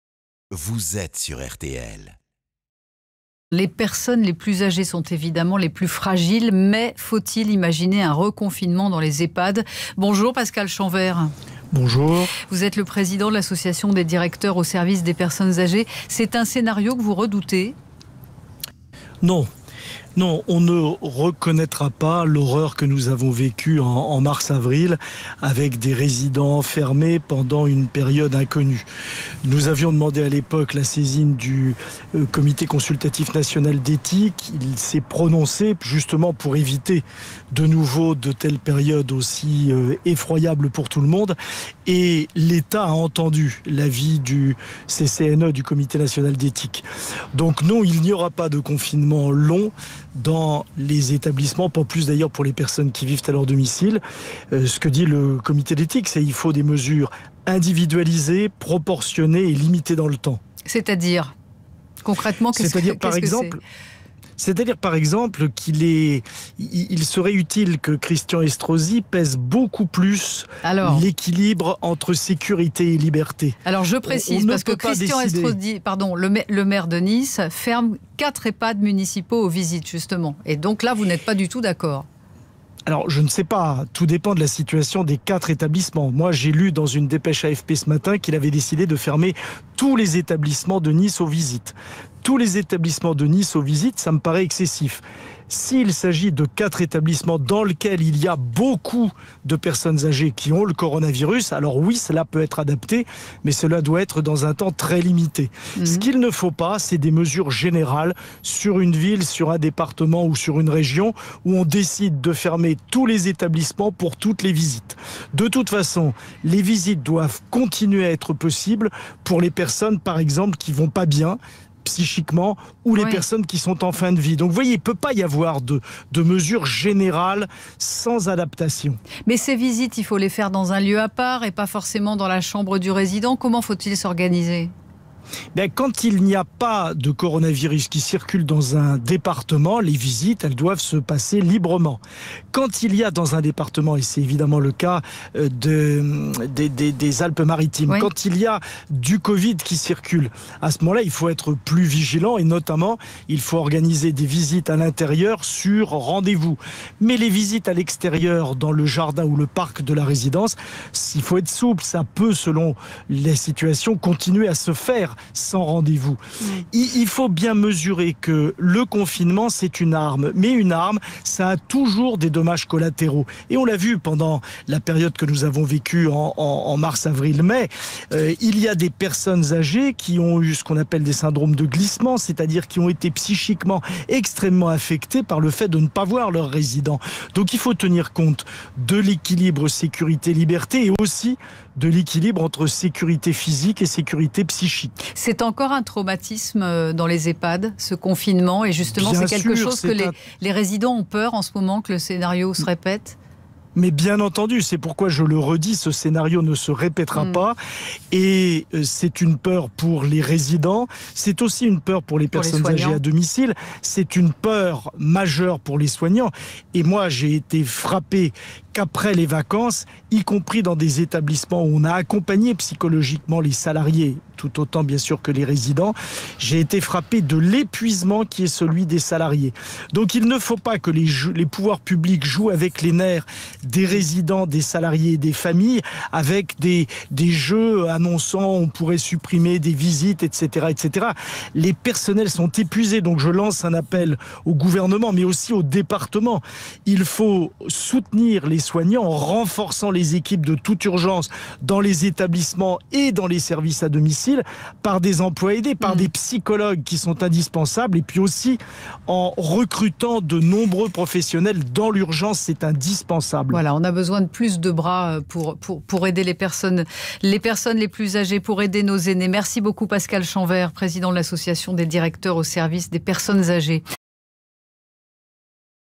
Ehpad : "On ne reconnaîtra pas l'horreur que nous avons vécu en mars-avril", assure Pascal Champvert
INVITÉ RTL - Un reconfinement est la crainte des personnes âgées, de leurs familles et des soignants, mais Pascal Champvert, président de l'Association des directeurs au service des personnes âgées, l'assure : ce scénario ne se reproduira pas.